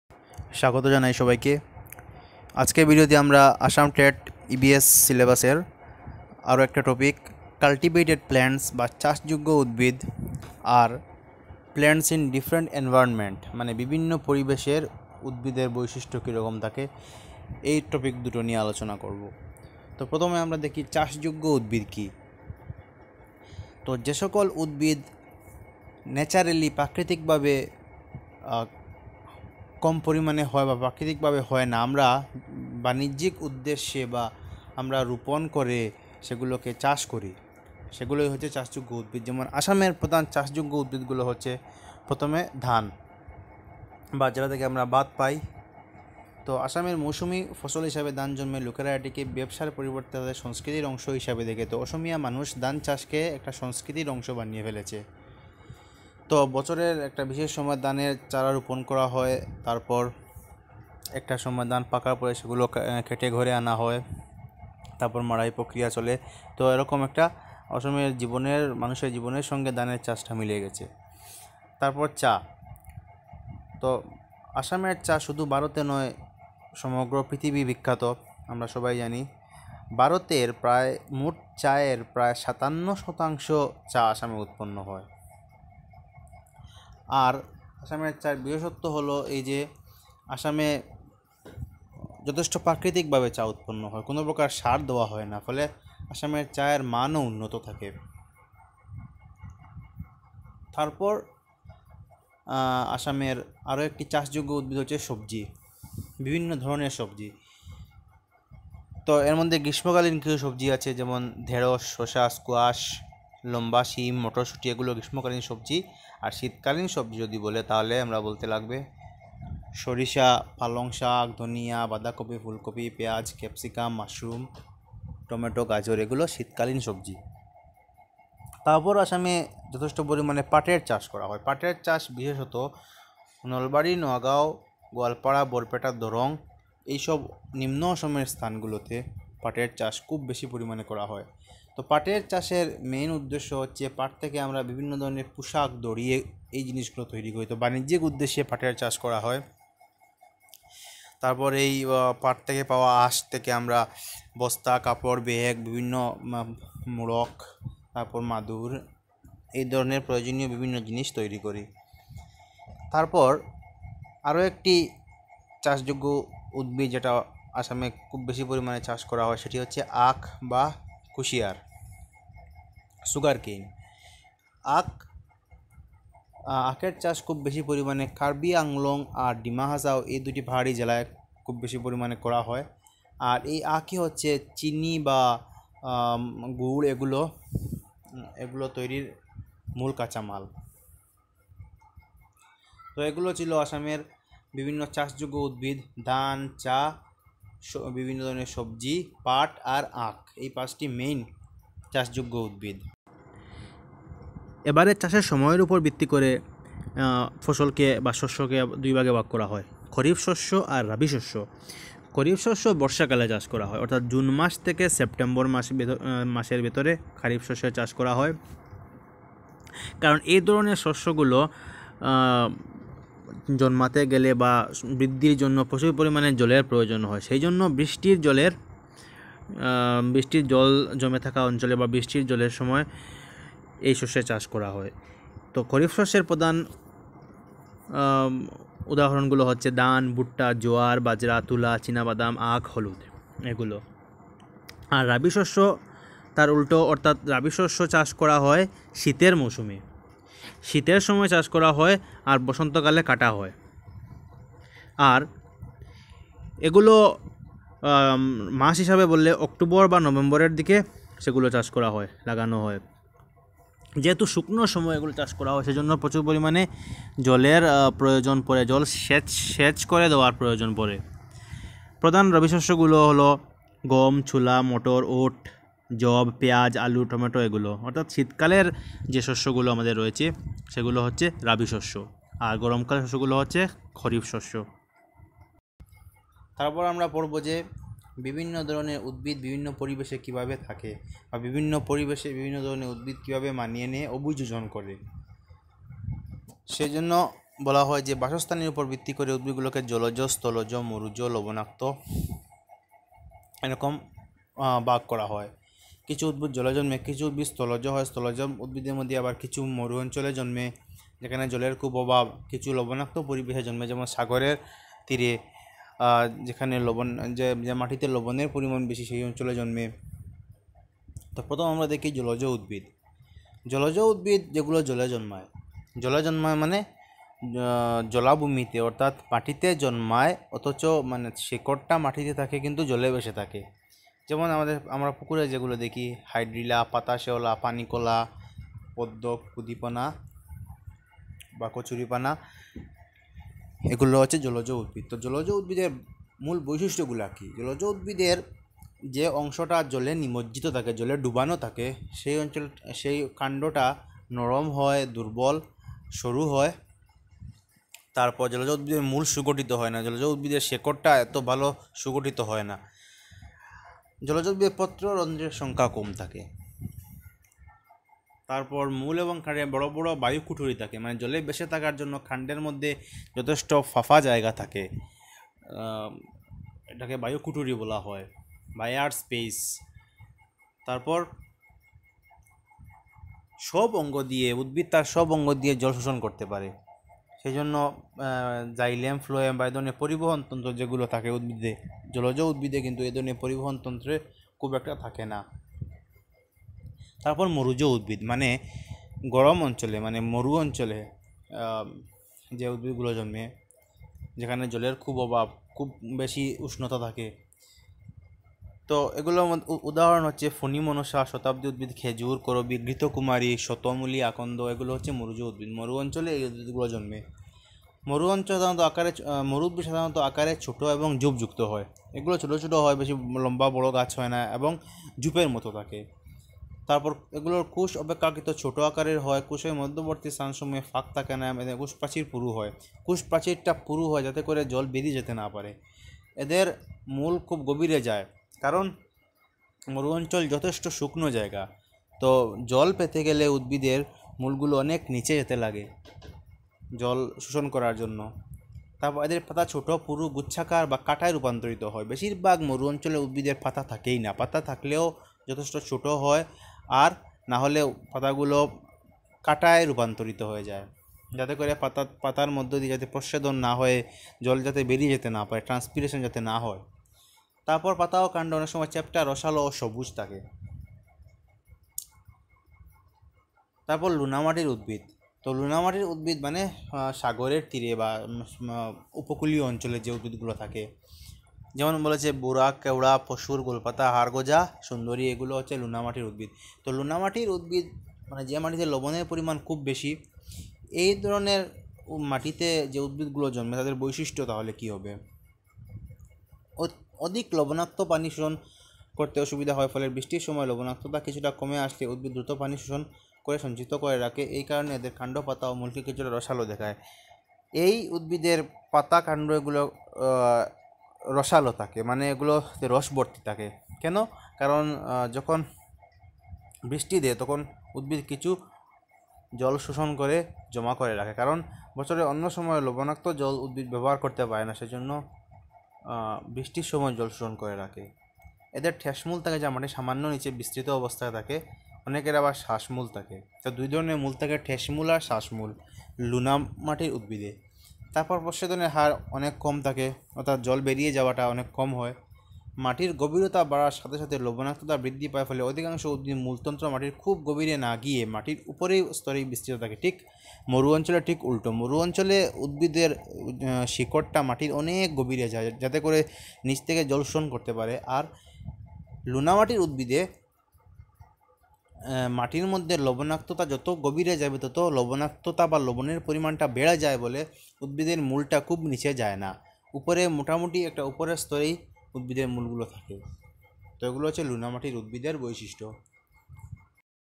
Class 17 | Cultivated Plants, Different env | চাষযোগ্য উদ্ভিদ ও বিভিন্ন পরিবেশের উদ্ভিদ | ATET EVS
Cultivated Plants and Plants Grown in Different environment
চাষযোগ্য উদ্ভিদ ও বিভিন্ন পরিবেশের উদ্ভিদ
Assam TET LP EVS Syllabus